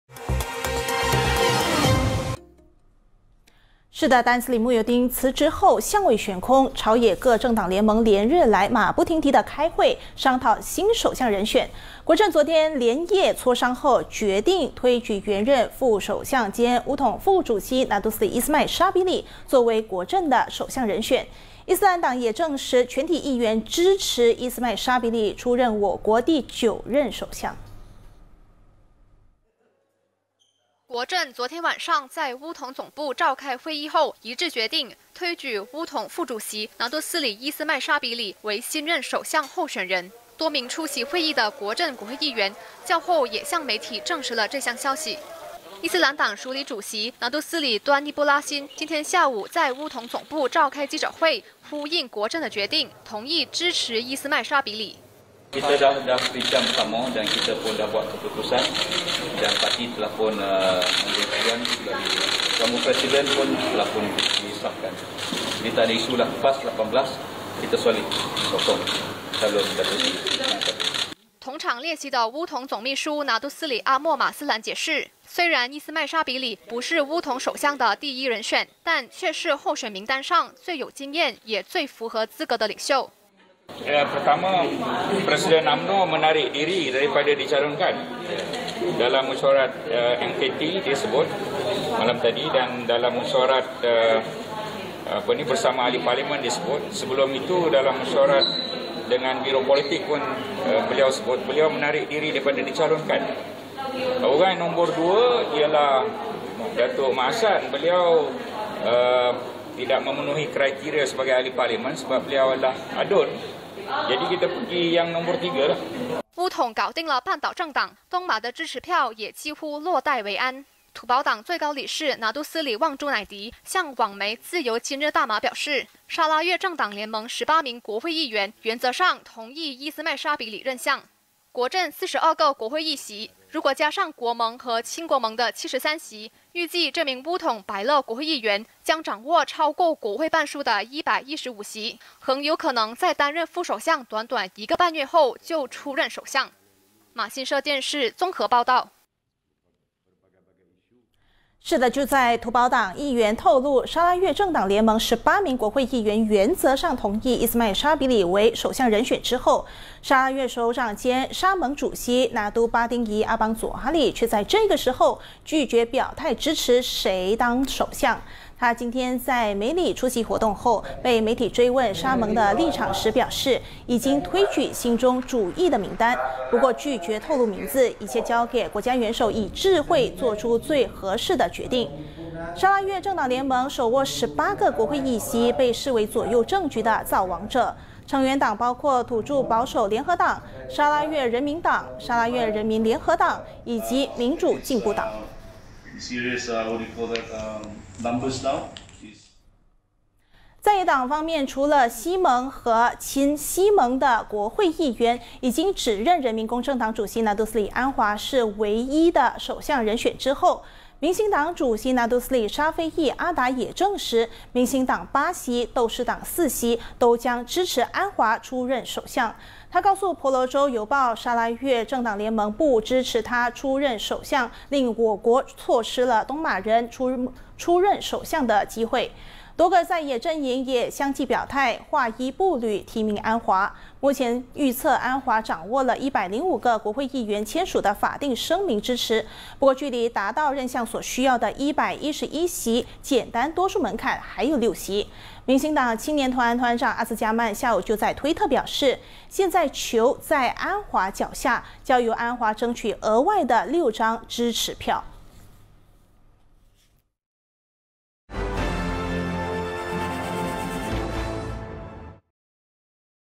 20210818 获国阵伊党同声推举 依斯迈沙比里任相在望
丹斯里慕尤丁辞职后，相位悬空，朝野各政党联盟连日来马不停蹄地开会商讨新首相人选，国阵昨天连夜磋商后，决定推举原任副首相兼巫统副主席拿督斯里依斯迈沙比里为国阵的首相人选，伊斯兰党也证实，全体议员支持依斯迈沙比里出任我国第九任首相。